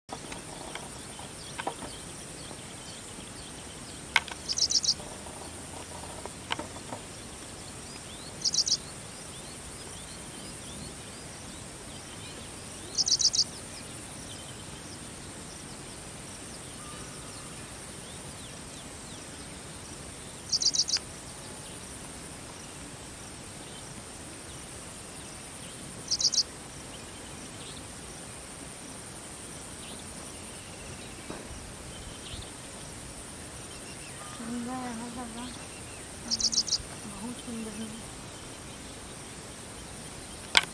Paddyfield Pipit Call with Spectrogram | Bengali Name মাঠচড়ুই বা তুলিকা | Bird Song 48
Paddyfield Pipit Call with Spectrogram

In this video, you'll be treated to the enchanting sound of the Paddyfield Pipit's call, which resonates through the air with a unique rhythm and tone. As you listen closely, you'll appreciate the bird's intricate vocalizations, rich in complexity and expression.
The Paddyfield Pipit's call is not only a delight to the ears but also serves important functions in its communication and territorial behavior. Through this video, you'll gain insights into the fascinating world of avian vocalizations and the significance they hold in the natural environment.

Paddyfield Pipit Call Paddyfield Pipit Song Paddyfield Pipit Sound